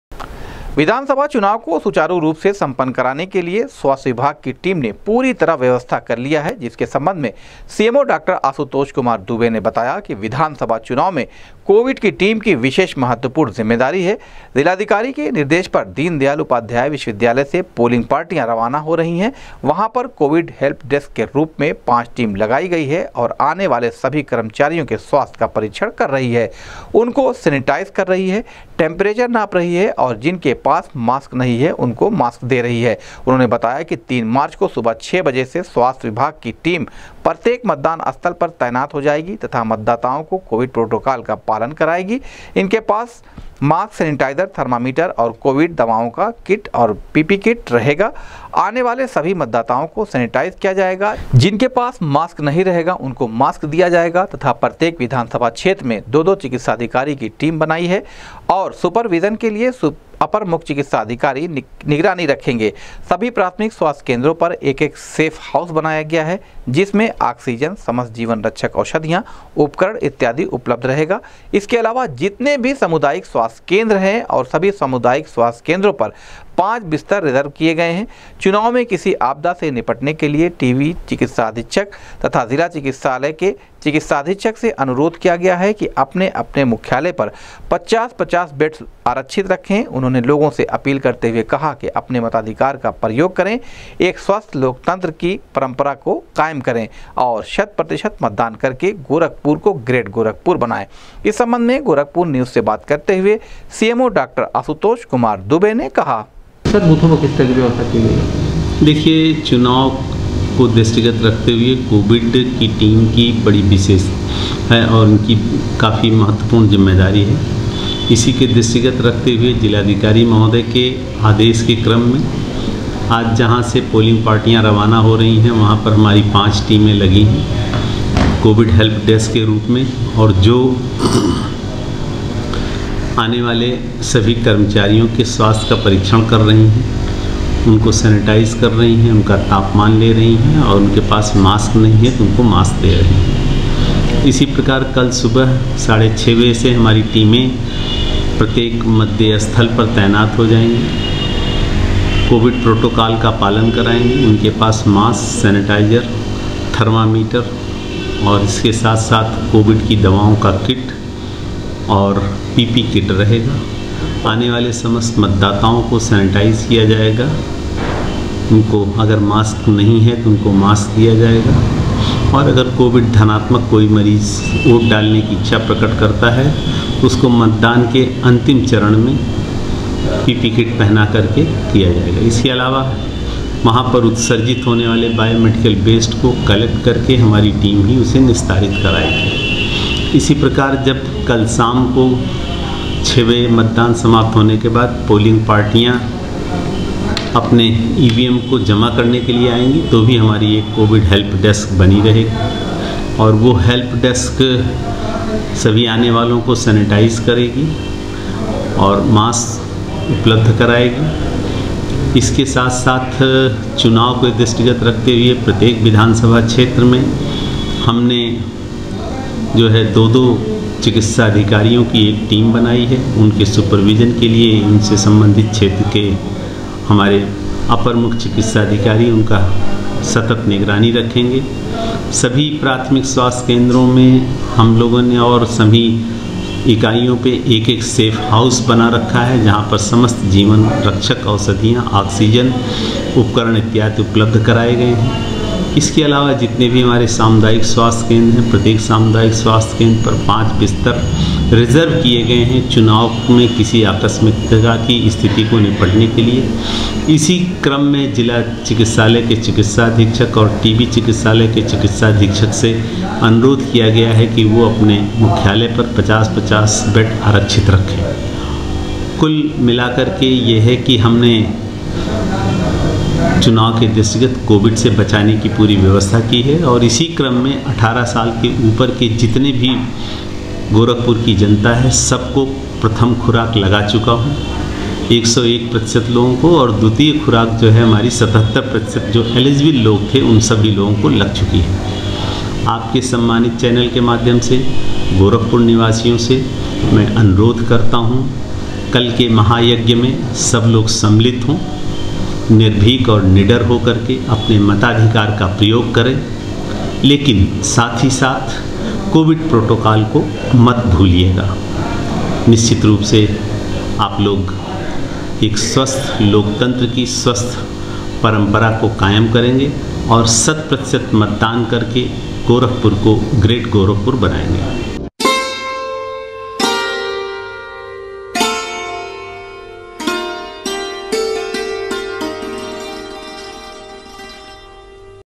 विधानसभा चुनाव को सकुशल संपन्न करवाने के लिए स्वास्थ्य विभाग की टीम पूरी तरह से है मुस्तैद, सीएमओ डॉ
विधानसभा चुनाव को सकुशल संपन्न करवाने के लिए स्वास्थ्य विभाग की टीम पूरी तरह से है मुस्तैद, सीएमओ डॉ आशुतोष कुमार दुबे ने कहा, प्रत्येक मतदान स्थल पर मतदाताओं को करायेंगी कोविड प्रोटोकॉल का पालन